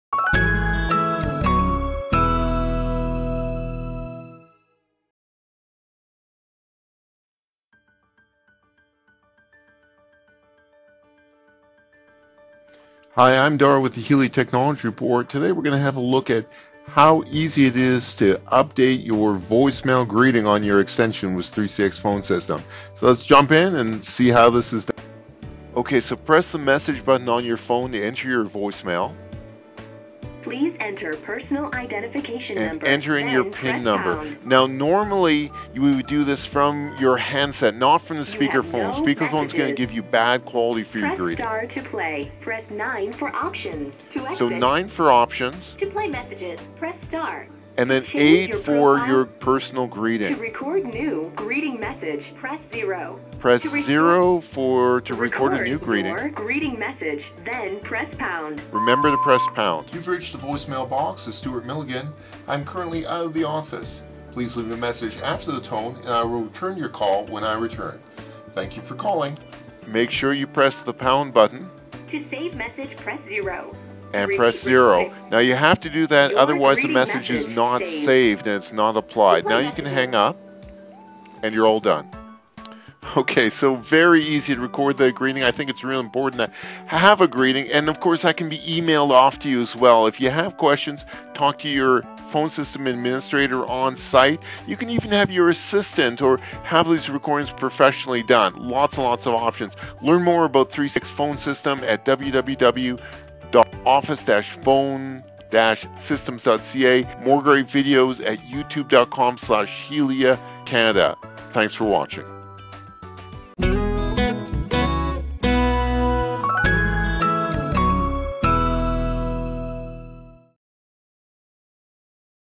How To Change Your Voicemail Greeting with 3CX Phone System
Easy Step by Step instructions on how to change your voicemail greeting on your desk phone.